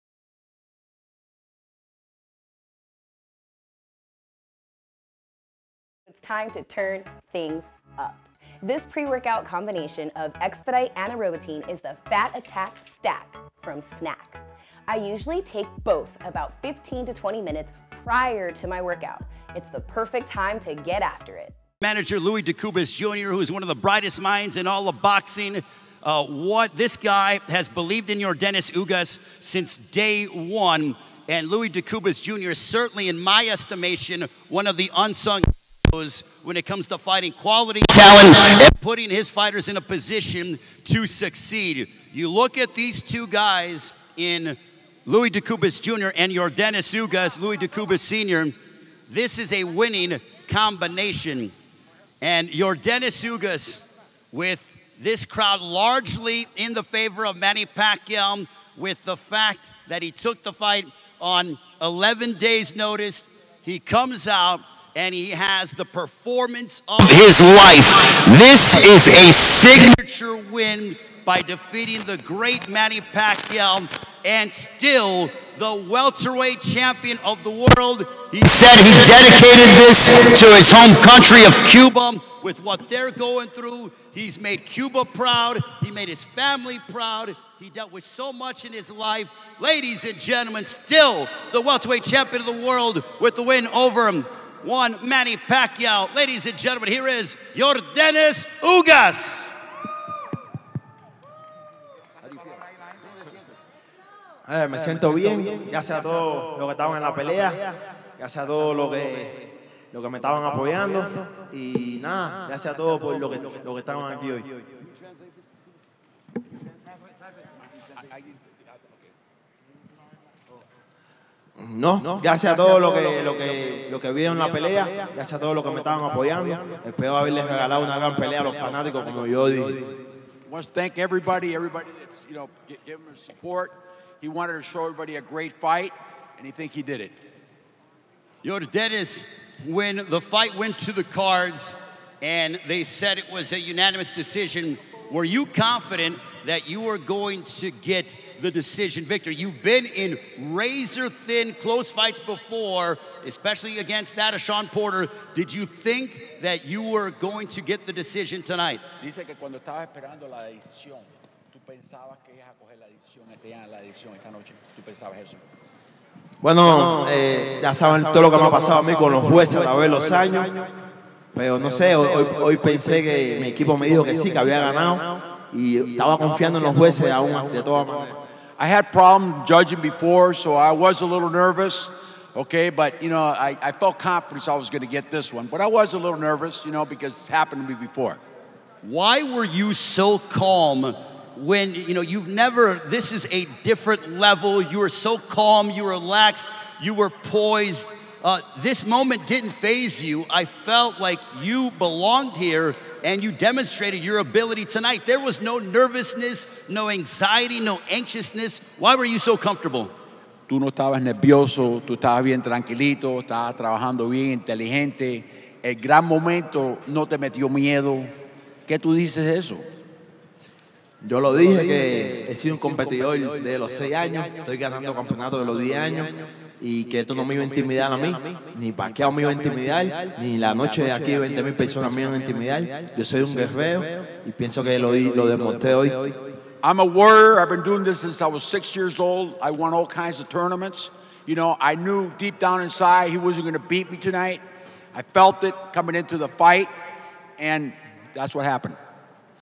YORDENIS UGAS REACTS TO UPSET VICTORY OVER MANNY PACQUIAO "I WASNT GONNA LET HIM INTIMIDATE ME!"
Yordenis Ugas reacts to his upset win over Manny Pacquiao! As we get his immediate reaction to upsetting Pacquioa as he says he is a warrior and that he wasnt going to let Pacquioa intimidate him in the Pacquiao Ugas fight